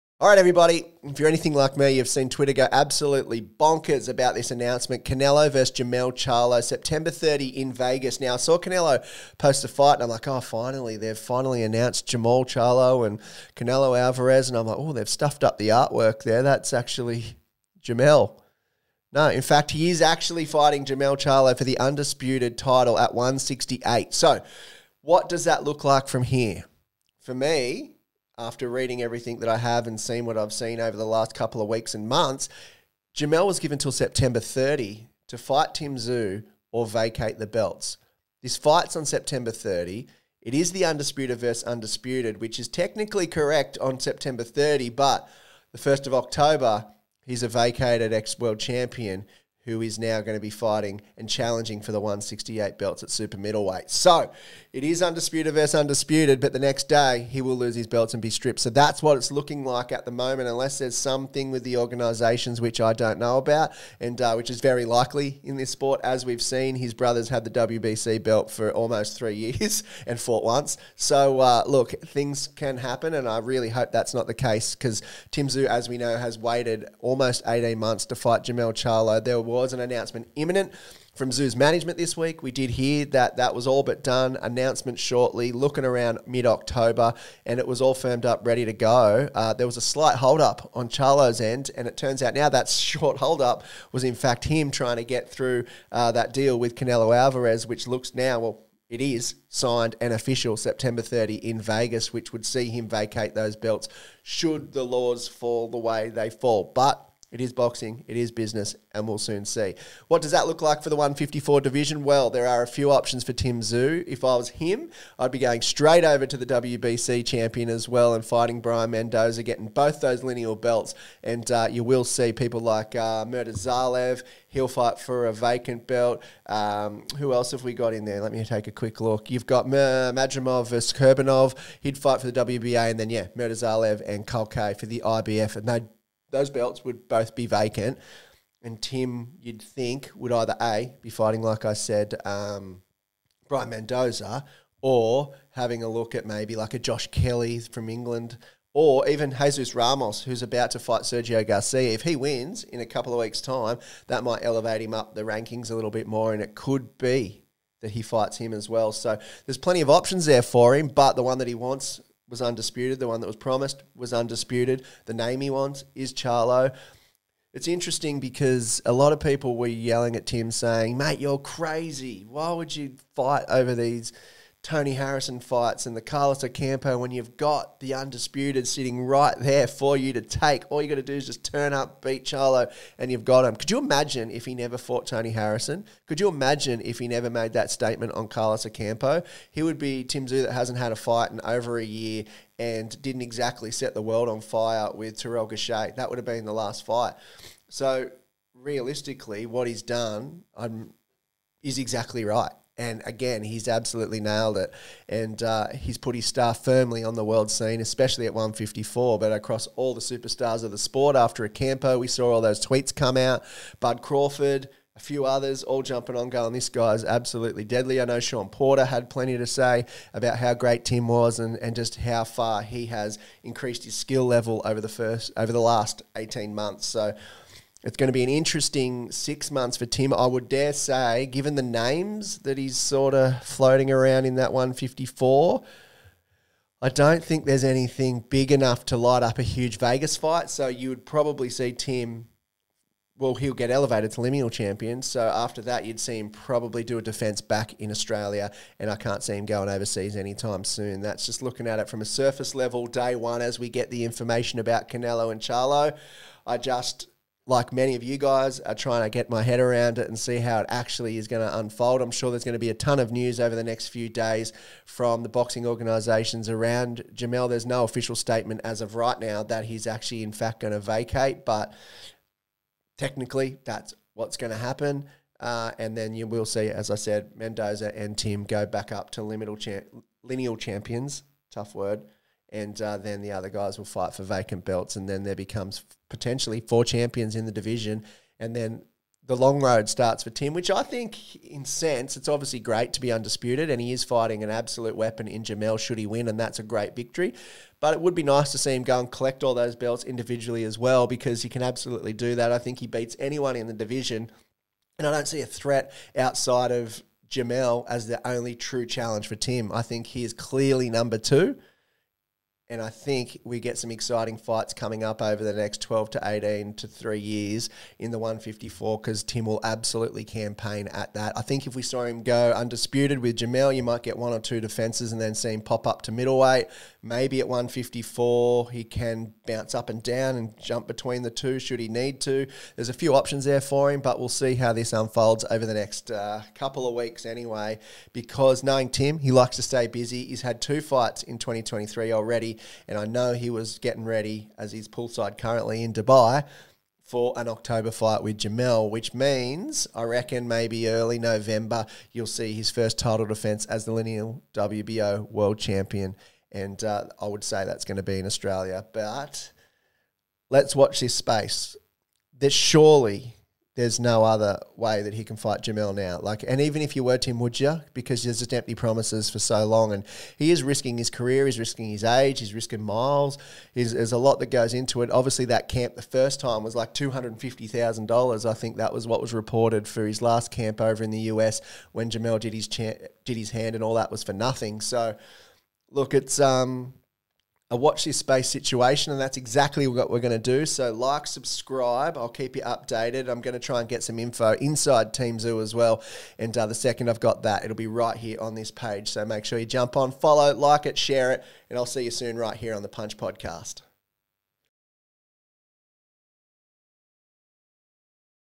Canelo V Charlo - What Now For Tszyu?
Announced today Canelo will fight Jermell Charlo in Vegas for his Undisputed belts at Super Middle Weight, Where does that leave Tim Tszyu who had all but secured his shot at all the marbles?